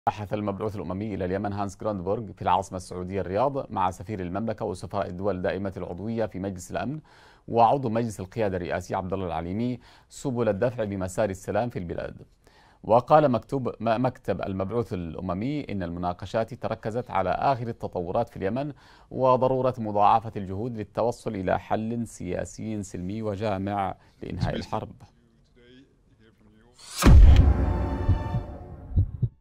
المبعوث الأممي يبحث في الرياض سبل الدفع بمسار السلام في اليمن
بحث المبعوث الأممي إلى اليمن هانس غروندبرغ في العاصمة السعودية الرياض

تليجرام